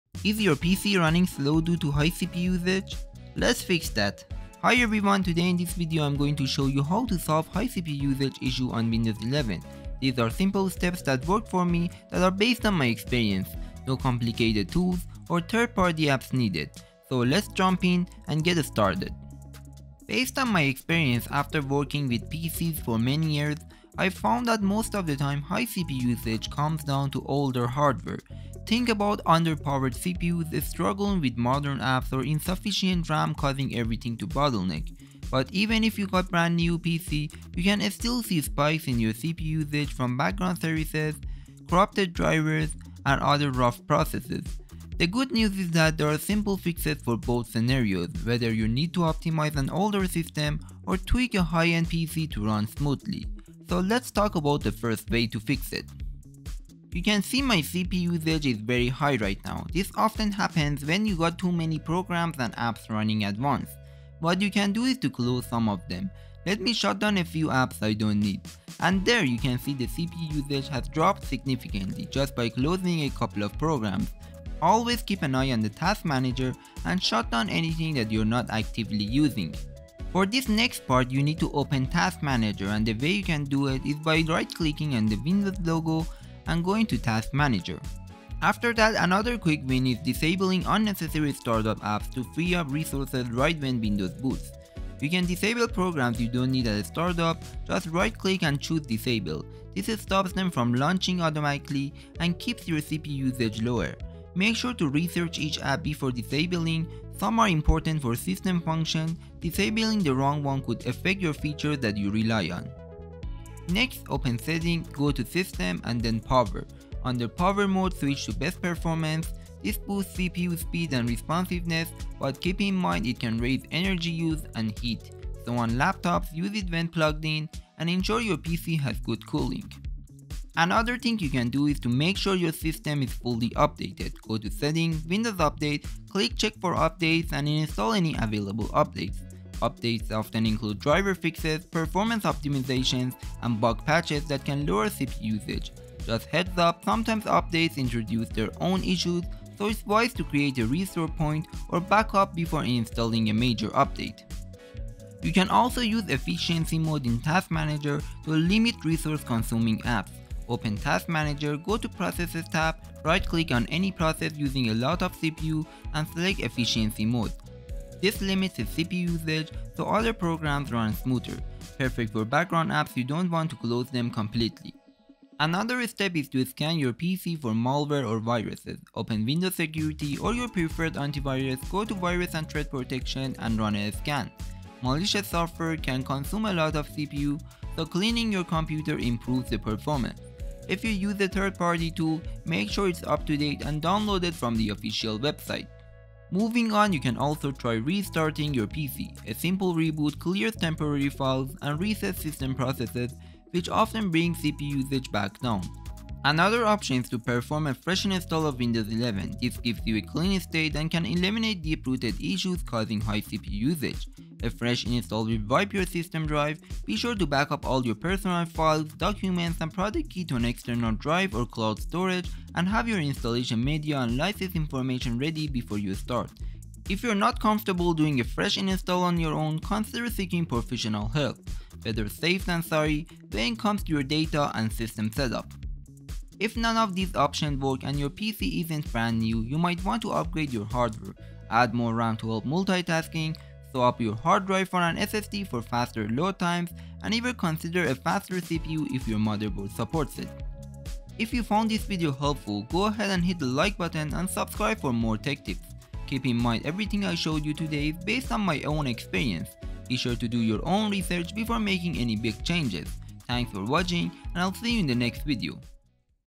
Fix 100% CPU Usage Windows 11 | Quick Fix 2025
Here is how to fix high CPU usage and boost performance in windows 11. The 100% CPU usage is a common issue in both PC and Laptop. CPU at 100 or CPU running at 100 can be fixed. Fix by Changing the power settings to balanced or High Performance. Play games without any lag with high performance by boosting FPS. Other methods to fix this can be seen in the video.